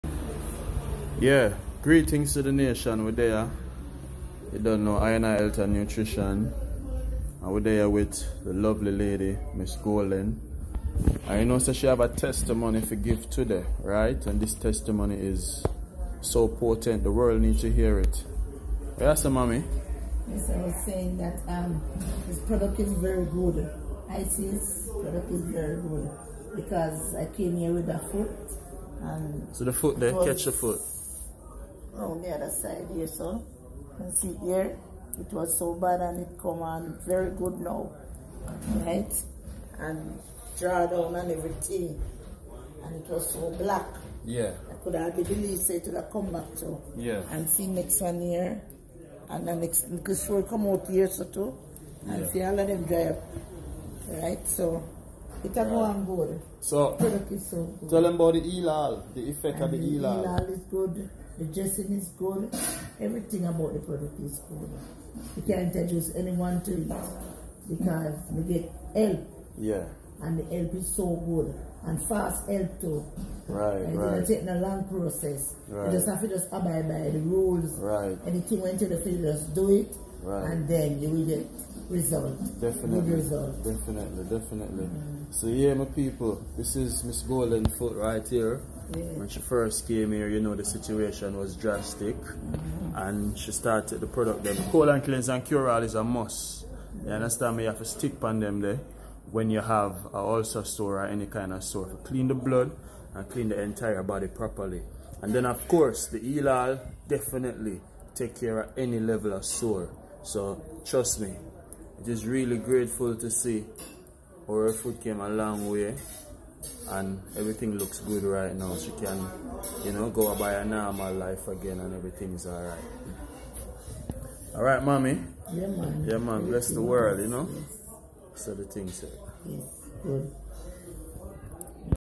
Poor Circulation And Ulcers No Match For Jamaican Bush Doctor
Golding
Royal Blessings And A Blissful Rise Worldwide 🌎🌍🌏 Mrs. Golding Suffered From Ulcered Feet And Poor Circulation. She Contacted Herbal Healer @kingisesi @inihealthandnutrition Took His Advice, Followed Protocol And The Rest Is History. She Is Indeed Happy With The Progress And Splendid Results So Far. Continue To Eat Healthy Exercise And Implement The Herbs For Healing My Nation Health Is Wealth! Contact Herbal Consultant @kingisesi Today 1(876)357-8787 Thank You!